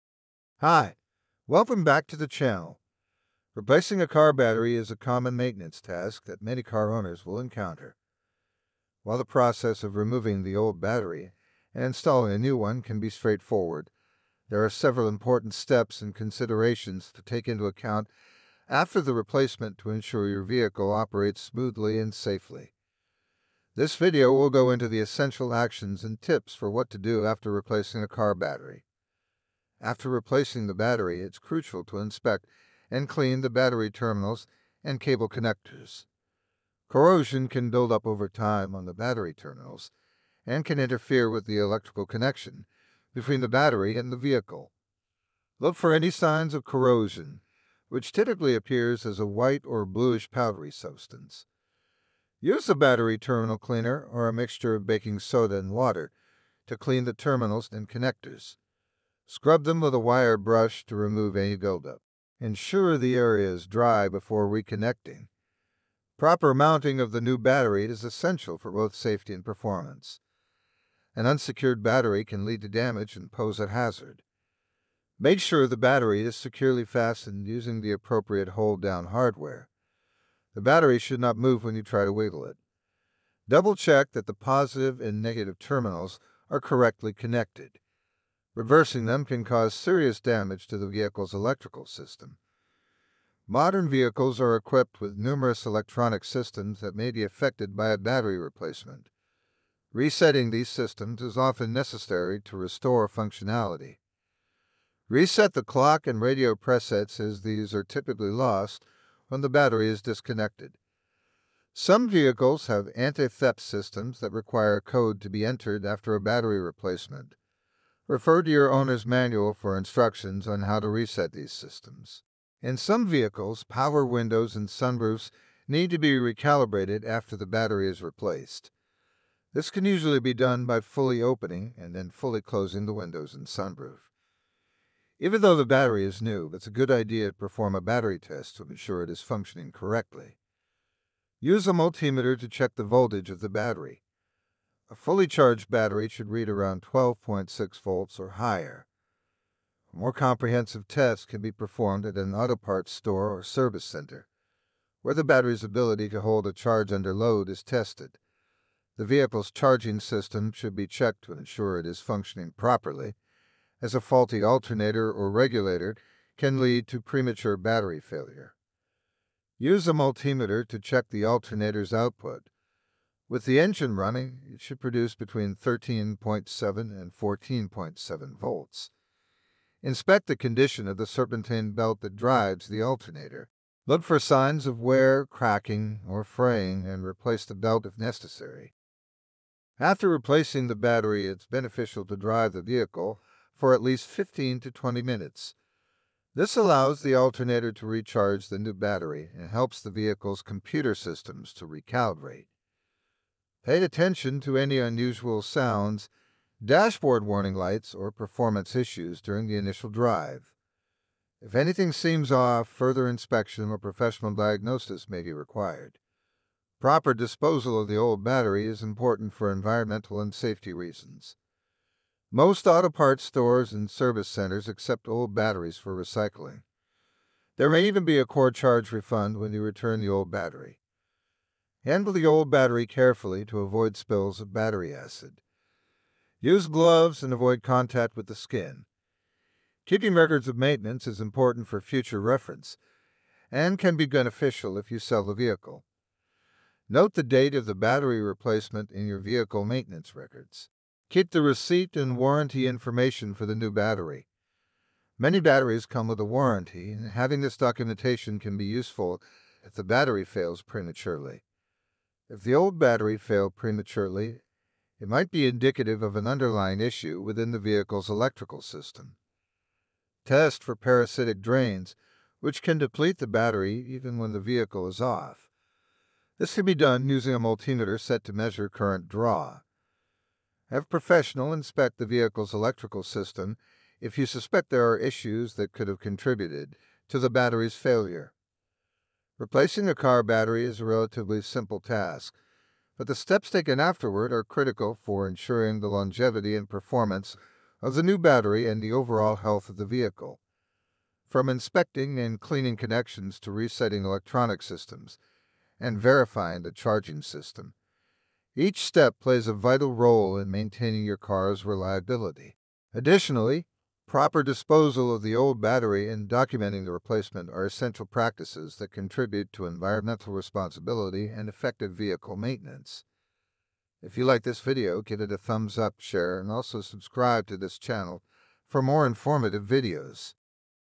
What to do after replacing car battery
Replacing a car battery is a relatively simple task, but the steps taken afterward are critical for ensuring the longevity and performance of the new battery and the overall health of the vehicle. From inspecting and cleaning connections to resetting electronic systems and verifying the charging system, each step plays a vital role in maintaining your car’s reliability. Additionally, properly disposing of the old battery and documenting the replacement are essential practices contributing to environmental responsibility and effective vehicle maintenance.